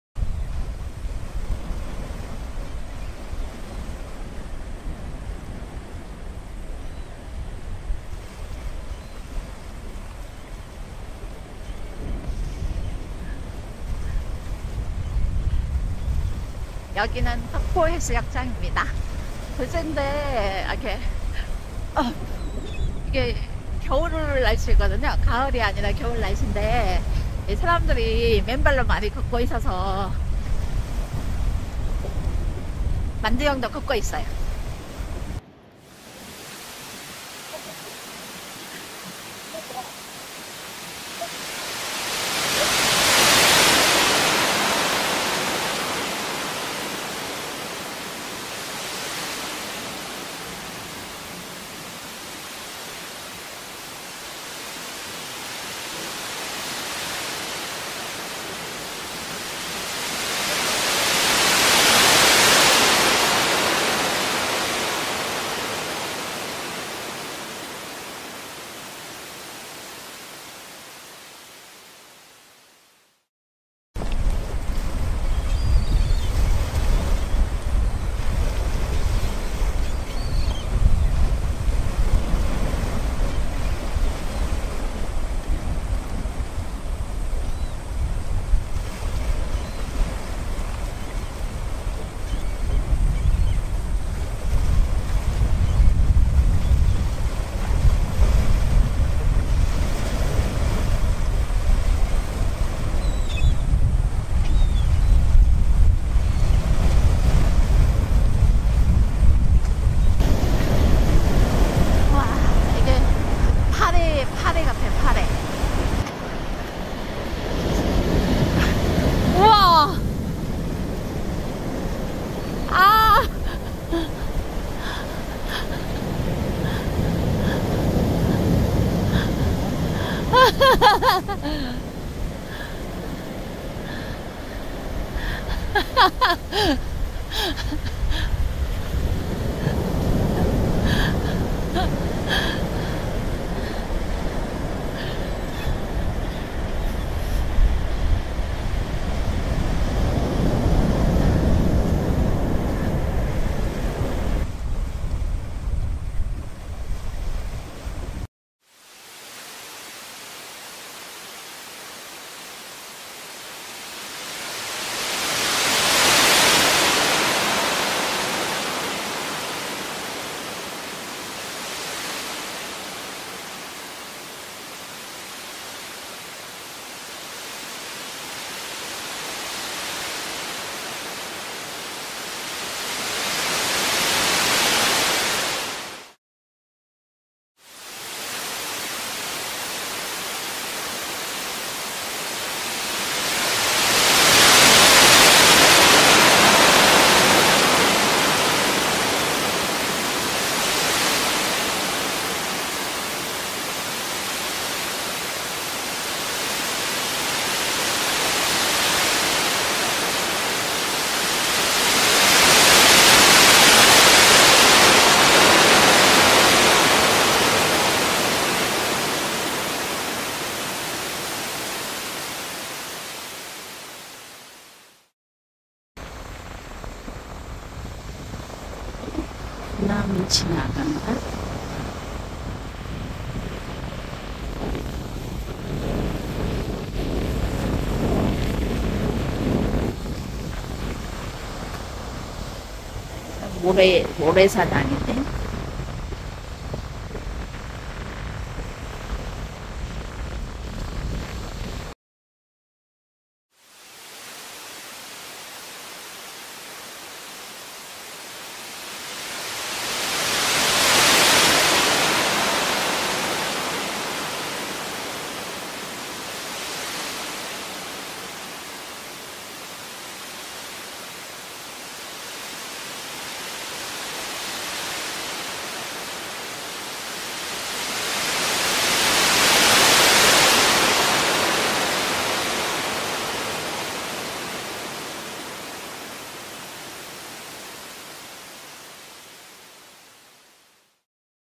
국내 12월 여행 지금 경남 거제 가볼만한곳 domestic winter travel
국내 12월 겨울 여행
domestic winter travel

여행을 좋아하는 요가사랑부부
겨울에 따뜻한 남도 거제 바다 여행을 다녀왔어요

거제 덕포해수욕장, 농소몽돌해수욕장, 유호전망대,구영해수욕장,황포해수욕장 다녀온 이야기를 영상에 담아 왔어요
자세한 이야기가 궁금하신 분들은 아래 블로그로 확인해주세요.

Manduhyeong Mukbang, eating show
💖  Please subscibe, U